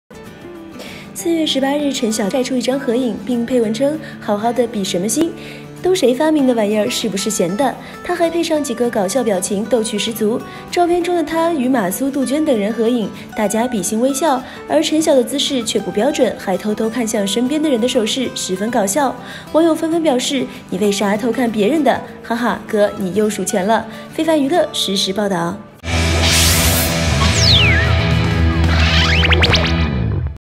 【2年前】陳曉與杜鵑等比心合影！手勢不標準被調侃“數錢”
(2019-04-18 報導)

4月18日，陳曉在自己微博曬出一張合影，并配文稱：“好好的比什么心，都誰發明的這玩意，是不是閑的？”